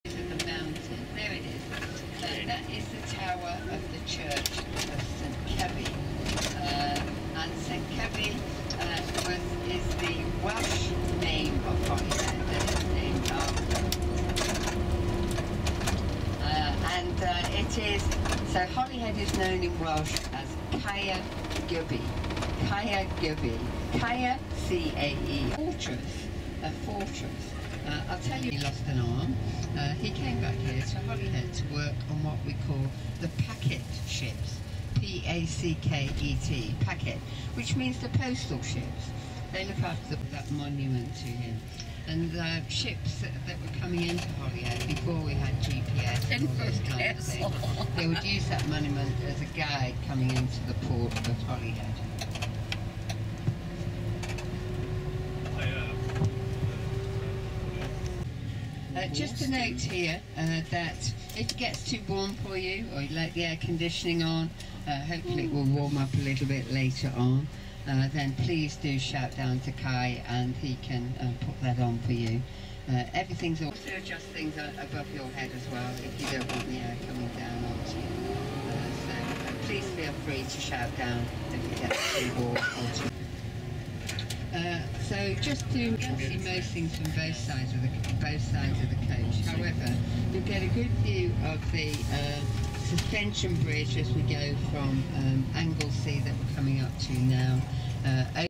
scenes and views of houses and other structures in Holyhead, Wales, UK on 5/22/23
These are what some of the houses and other structures look like in the Welsh town of Holyhead in the U.K. I got this footage from a Viking cruises hired bus while we were in Holyhead where we disembarked at the commercial port and were heading from there all the way to Porthmadog to ride the Ffestiniog heritage narrow gauge railway.